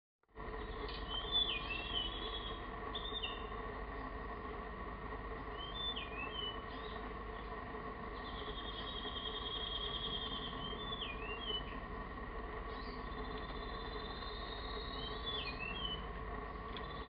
Singing male Mangrove Blue Flycatcher
Mangrove Blue Flycatcher vocalising from its favourite perch one afternoon. A rare resident on an off shore island.